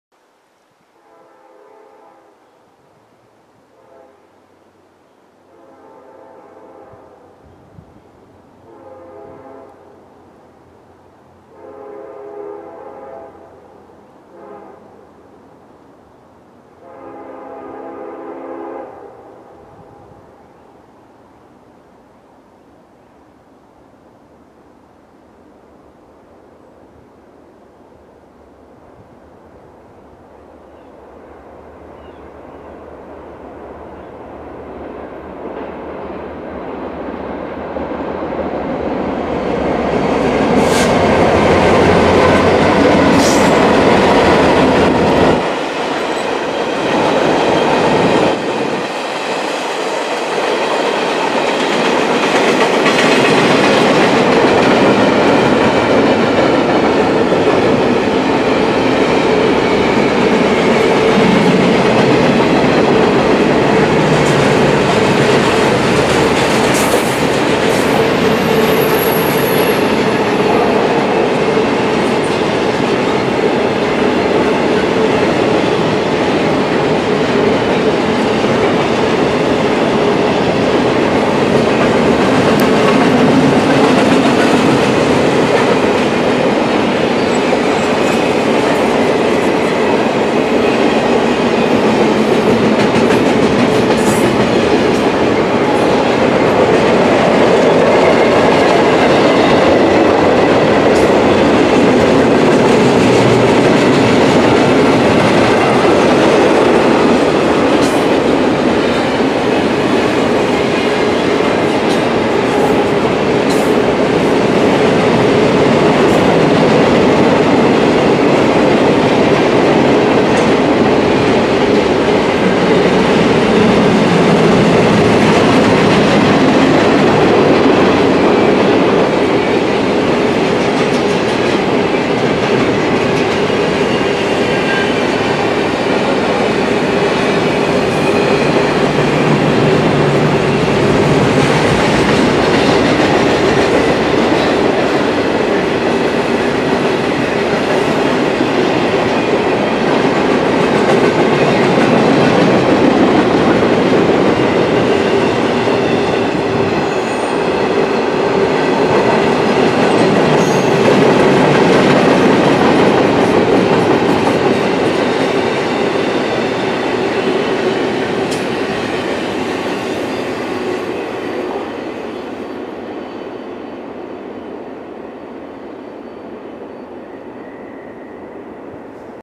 NS 2726, NS 9961, and NS 2531 in the Woods in Delphi, Indiana (April 2016)
While exploring the former interurban right-of-way in Delphi, Indiana, I heard this train coming from the distance. This is recorded in the woods east of the Norfolk Southern tracks and west of Hamilton Street. This is an Intermodal stack train and it is heading south. Norfolk Southern units 2726 (SD70M-2), 9961 (C44-9W), and 2531 (SD70) are leading the train. This is the NS Lafayette District.